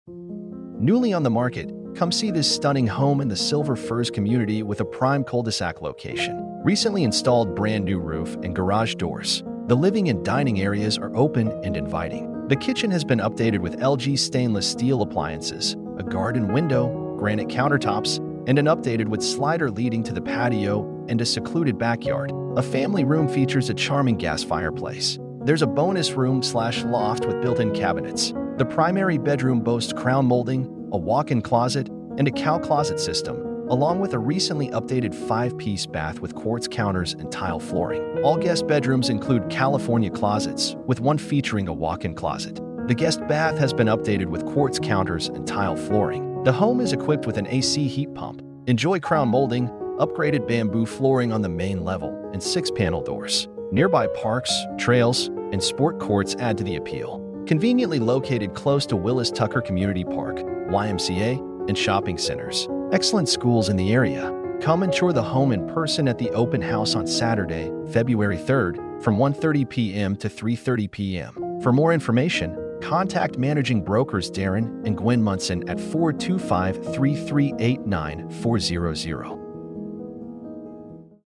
15104 51st Dr SE, Everett, WA 98208 listed by Darren & Gwen Munson
Newly on the market, come see this stunning home in the Silver Firs Community with a Prime Cul-de-sac Location.

Recently installed brand new roof and garage doors. The living and dining areas are open and inviting. The kitchen has been updated with LG stainless steel appliances, a garden window, granite countertops, and an updated wood slider leading to the patio and a secluded backyard. A family room features a charming gas fireplace. There's a bonus room/loft with built-in cabinets. The primary bedroom boasts crown molding, a walk-in closet, and a Cal Closet system, along with a recently updated 5-piece bath with quartz counters and tile flooring. All guest bedrooms include California closets, with one featuring a walk-in closet. The guest bath has been updated with quartz counters and tile flooring. The home is equipped with an AC heat pump. Enjoy crown molding, upgraded bamboo flooring on the main level, and 6-panel doors. Nearby parks, trails, and sport courts add to the appeal. Conveniently located close to Willis Tucker Community Park, YMCA, and shopping centers. Excellent schools in the area!

Come and tour the home in person at the open house on Saturday, February 3, from 1:30 pm to 3:30 pm.

🏡15104 51st Dr SE, Everett, WA 98208
Listing Price $870,000 | 4 BED | 3 BATH | 2,222 SQFT | MLS # 2193216

📲 Darren and Gwen Munson
MANAGING BROKERS